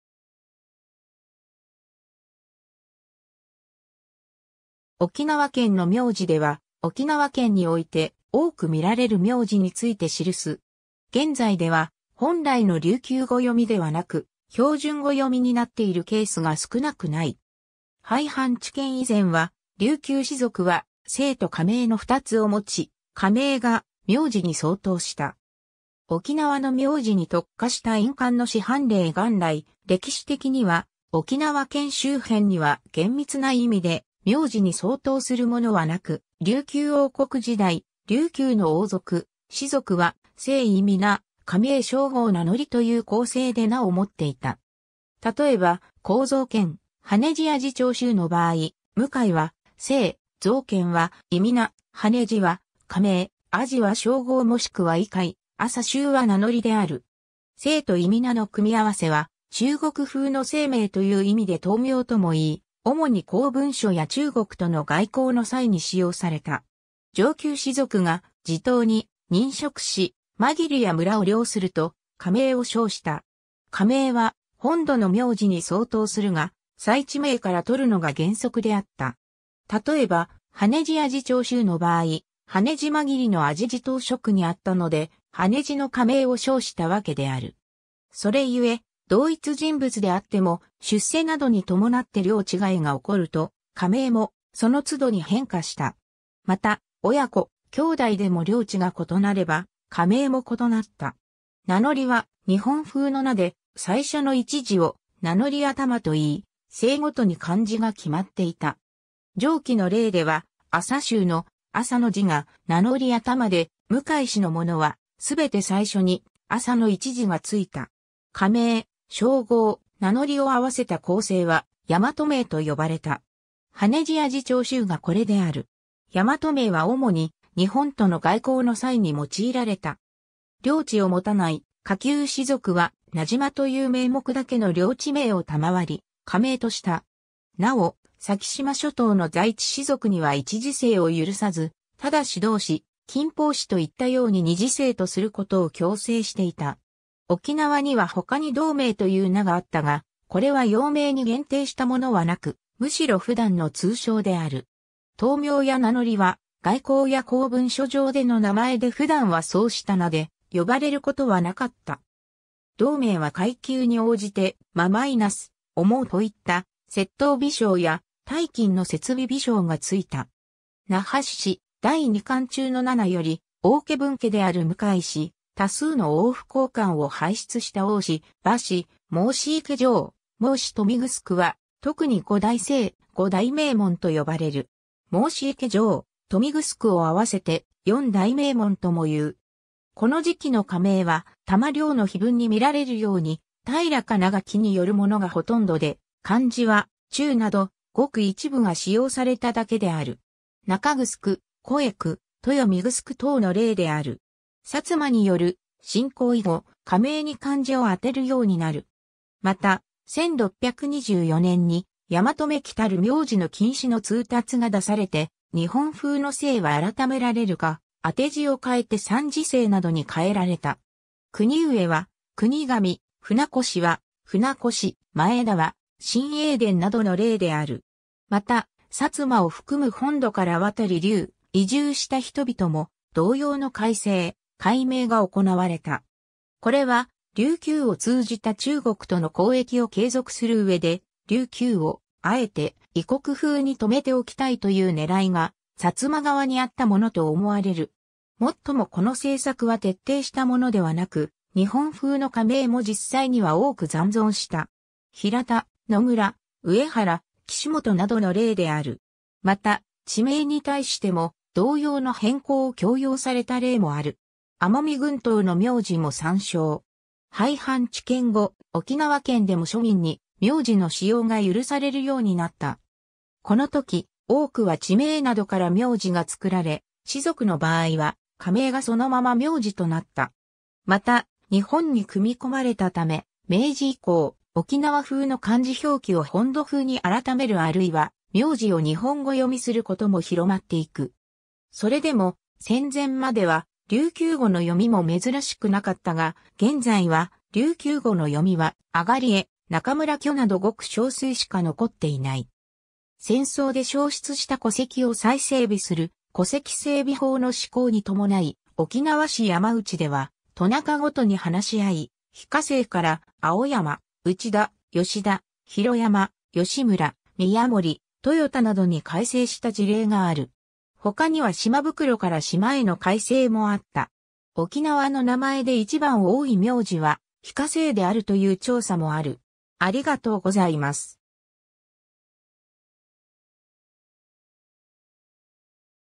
沖縄県の名字
沖縄県の名字（おきなわけんのみょうじ）では、沖縄県において多く見られる名字について記す。
現在では本来の琉球語（琉球方言）読みではなく、標準語読みになっているケースが少なくない。
廃藩置県以前は、琉球士族は姓と家名（かめい、琉球方言:屋の名 ヤーヌナー,ヤーンナー）の2つを持ち、家名が名字に相当した。
 沖縄の名字に特化した印鑑の市販例 元来、歴史的には沖縄県周辺には厳密な意味で、名字に相当するものはなく、琉球王国時代、琉球の王族・士族は姓（氏）＋諱、家名＋称号＋名乗という構成で名を持っていた。
例えば、向象賢・羽地按司朝秀の場合、「向」は姓、「象賢」は諱、「羽地」は家名、「按司」は称号もしくは位階、「朝秀」は名乗である。
姓と諱の組み合わせは中国風の姓名という意味で唐名（からな）とも言い、主に公文書や中国との外交の際に使用された。
 上級士族が地頭に任職し、間切や村を領すると、家名（ヤーンナー）を称した。
家名は本土の名字に相当するが、采地名（領地名）から採るのが原則であった。
例えば、羽地按司朝秀の場合、羽地間切の按司地頭（総地頭）職にあったので、羽地の家名を称したわけである。
それゆえ、同一人物であっても、出世などに伴って領地替えが起こると、家名もその都度に変化した。
また、親子、兄弟でも領地が異なれば、家名も異なった。
名乗（なのり）は日本風の名で、最初の一字を名乗頭（なのりがしら）と言い、姓ごとに漢字が決まっていた。
上記の例では、朝秀の「朝」の字が名乗頭で、向氏（王家子孫）の者は、すべて最初に「朝」の一字が付いた。
家名・称号・名乗を合わせた構成は、大和名（やまとな）と呼ばれた。
羽地按司朝秀がこれである。
大和名は主に日本との外交の際に用いられた。
 領地を持たない下級士族は、名島（なじま）という名目だけの領地名を賜り、家名とした。
 なお、先島諸島の在地士族には1字姓を許さず、忠導氏、錦芳氏といったように2字姓とすることを強制していた。
 沖縄には他に童名（ドーナ、ワラビナー）という名があったが、これは幼名に限定したものはなく、むしろ普段の通称である。
唐名や名乗は外交や公文書上での名前で普段はそうした名で呼ばれることはなかった。
童名は階級に応じて、「真-」「思-」といった接頭美称や「-金」の接尾美称が付いた。
 ※『那覇市史』第2巻中の7より 王家分家である向氏、多数の王府高官を輩出した翁氏、馬氏、毛氏池城、毛氏豊見城は、特に五大姓（氏）、五大名門と呼ばれる。
毛氏池城・豊見城を合わせて、四大名門とも言う。
 この時期の家名は、玉陵の碑文（1501年）に見られるように、平仮名書き（一部片仮名書き）によるものがほとんどで、漢字は「中」など、ごく一部が使用されただけである。
「中ぐすく（中城）」、「こゑく（越来）」、「とよミぐすく（豊見城）」等の例である。
 薩摩による侵攻以後、家名に漢字を充てるようになる。
また、1624年に「大和めきたる名字の禁止」の通達が出されて、日本風の姓は改められるか、当て字を換えて三字姓などに変えられた。
「国上」は「国頭」、「船越」は「富名腰」、「前田」は「真栄田」などの例である。
また薩摩を含む本土から渡琉、移住した人々も同様の改姓・改名が行われた。
これは琉球を通じた中国との交易を継続する上で、琉球をあえて“異国風”に留めておきたいという狙いが薩摩側にあったものと思われる。
もっともこの政策は徹底したものではなく、日本風の家名も実際には多く残存した。
「平田」「野村」「上原」「岸本」などの例である。
また、地名に対しても同様の変更を強要された例もある。
奄美群島の名字も参照。
 廃藩置県後、沖縄県でも庶民に名字の使用が許されるようになった。
この時、多くは地名などから名字が作られ、士族の場合は、家名がそのまま名字となった。
また、日本に組み込まれたため、明治以降、沖縄風の漢字表記を本土風に改める（冨名腰→船越など）あるいは名字を日本語読みする（喜屋武「チャン」→「キャン、キヤタケ」、大城「ウフグシク、ウフグスク」→「オオシロ」、島袋「シマブク」→「シマブクロ」など）ことも広まっていく。
それでも戦前までは琉球語（琉球方言）の読みも珍しくなかったが、現在は、琉球語の読みは 東江（アガリエ）、仲村渠（ナカンダカリ、ナカンラカリ）などごく少数しか残っていない。
 戦争で消失した戸籍を再整備する「戸籍整備法」の施行に伴い、沖...